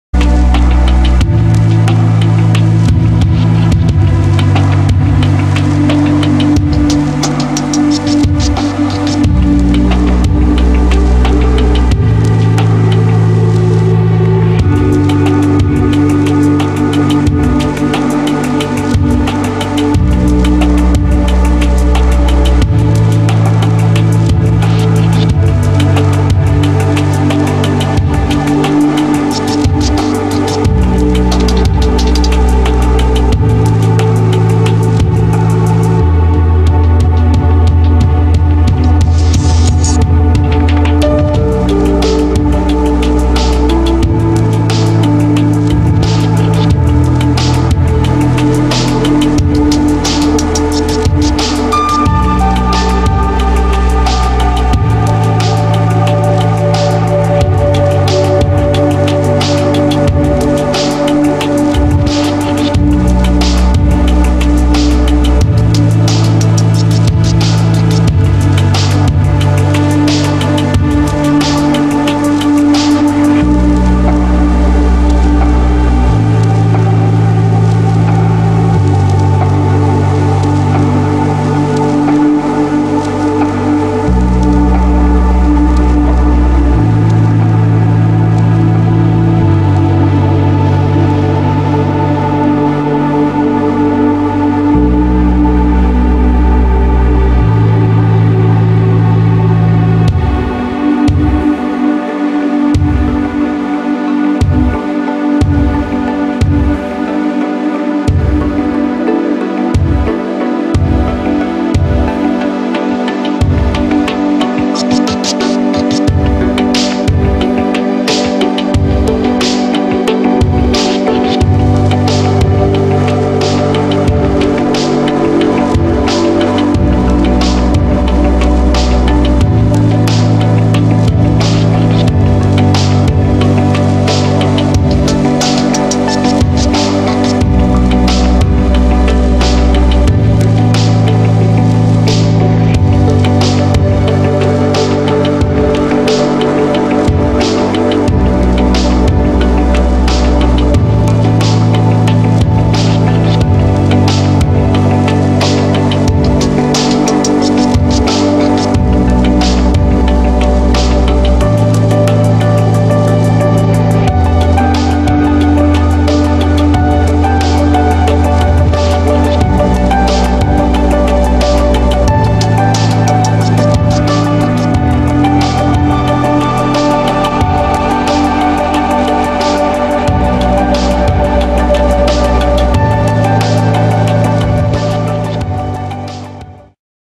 barra de mira-poco da cruz aveiro portugal 4k aerial drone
aldeia barra de mira (poco da cruz)aveiro portugal.pesca,agricoltura,floresta...muita batata lol